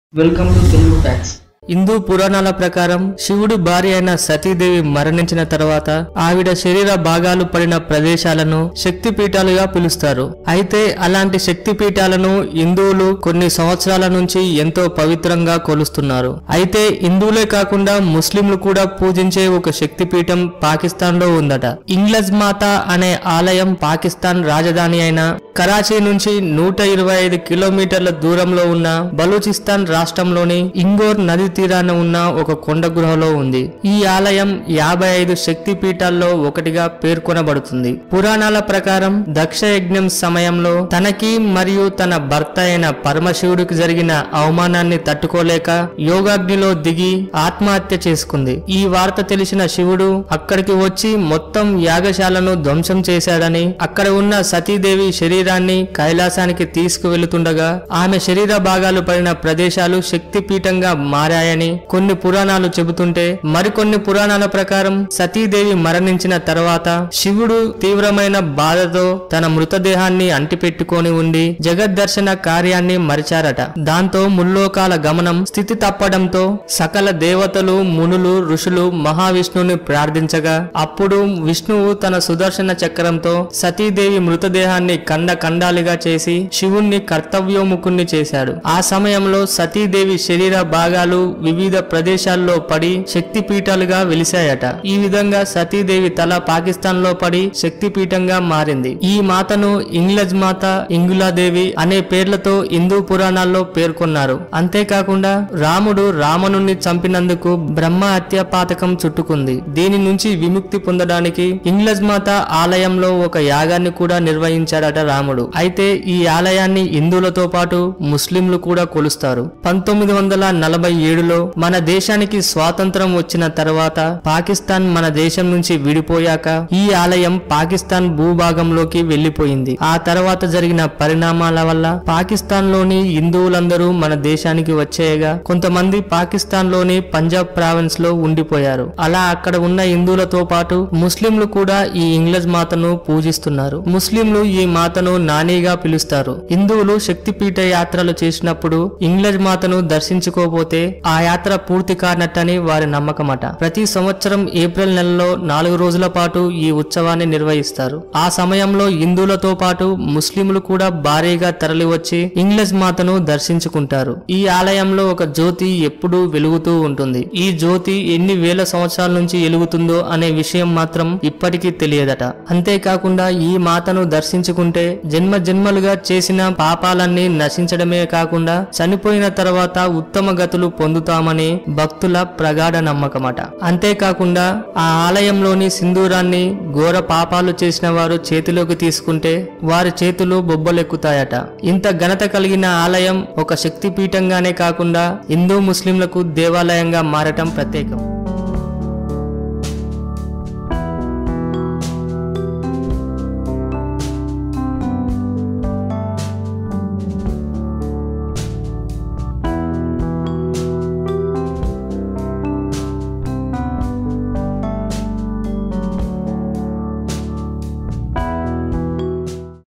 పాకిస్తాన్ లో ఉన్నశక్తి పీఠం గురించి తెలుసా? || Hinglaj Devi Temple in Pakistan || Telugu Facts
Hinglaj Mata, also known as Hinglaj Devi, Hingula Devi and Nani Mandir, is a Hindu temple in Hinglaj, a town on the Makran coast in the Lasbela district of Balochistan, Pakistan, and is the middle of the Hingol National Park. It is one of the Shakti Peethas of the goddess Sati. It is a form of Durga or Devi located in a mountain cavern on the banks of the Hingol River.